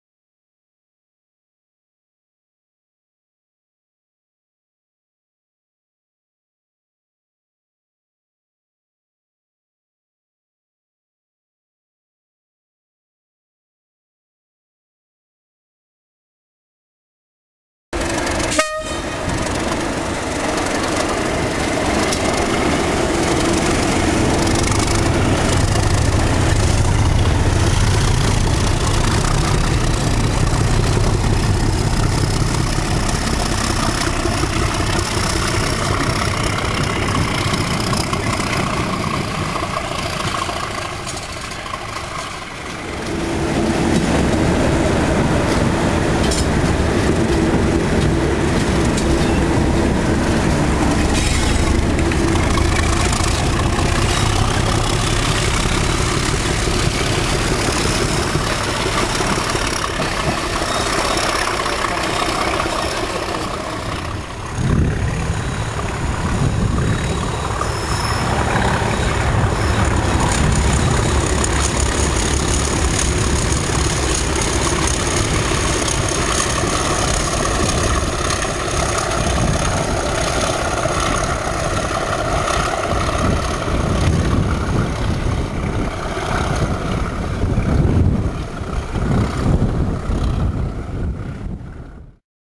37688 & 37682 on a second 6M95 this week, 25/06/09
37688 "Kingmoor TMD" & 37682 are seen at Midley Crossing (between Lydd & Brenzett), Snargate Crossing (near Appledore) and Appledore, working a second 6M95 Dungeness - Willesden Brent flasks this week. 25/06/09